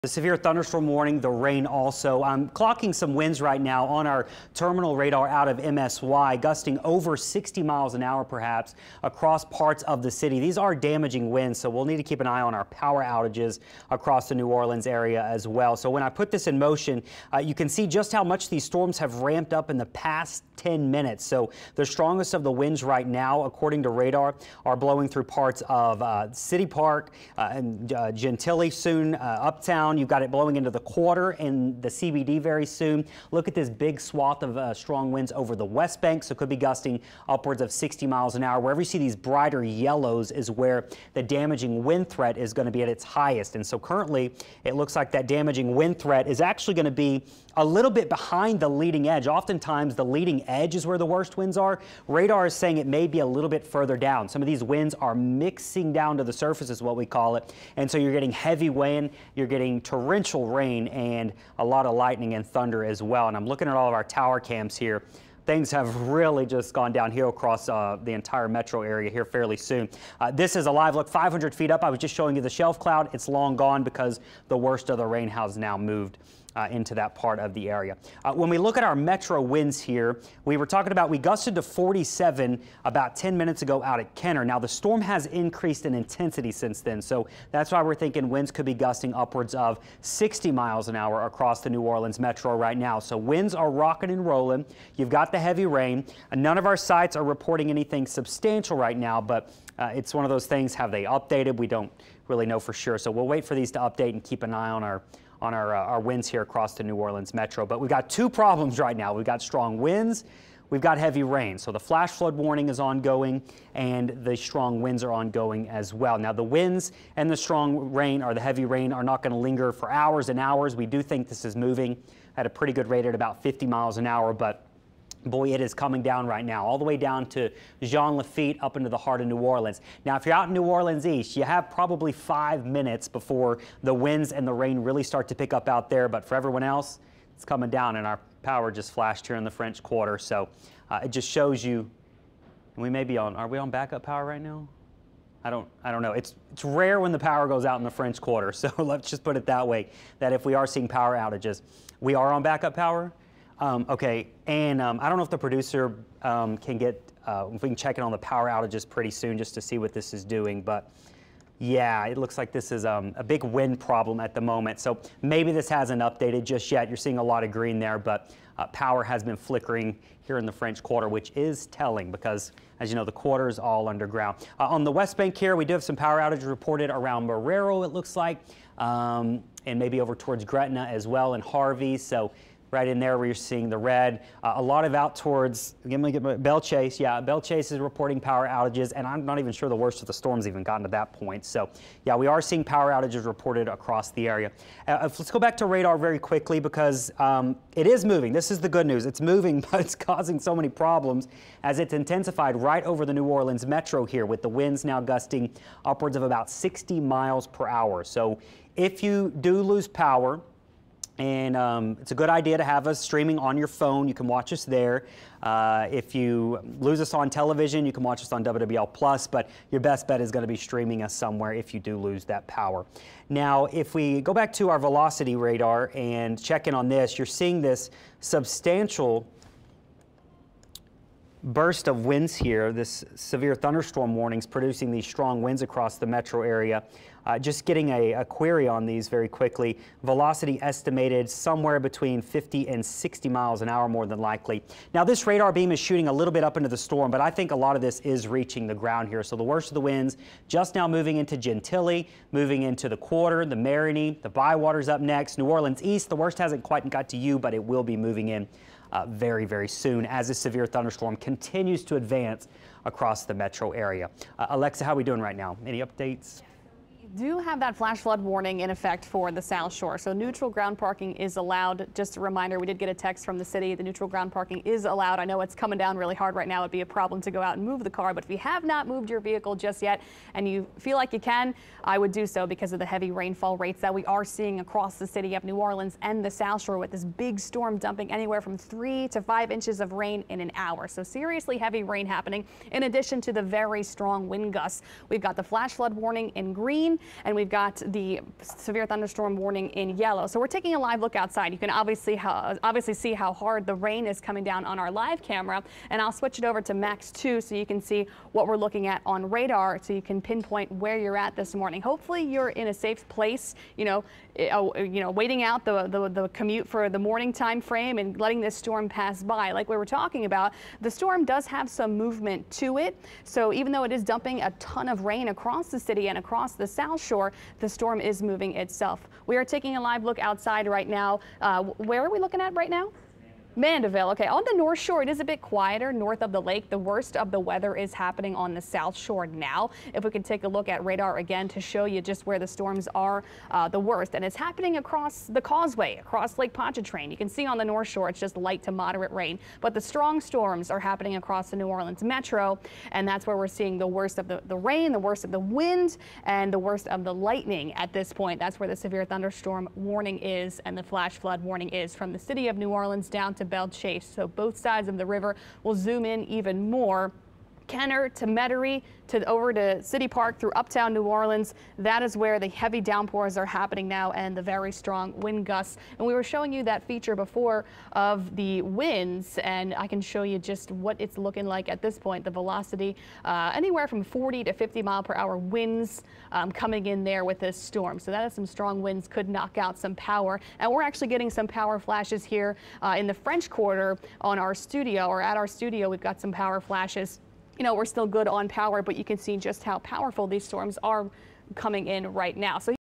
Tracking Storms in Southeast Louisiana: 9AM Weather Coverage
Meteorologist Payton Malone has the latest updates amid severe thunderstorms and what to watch out for.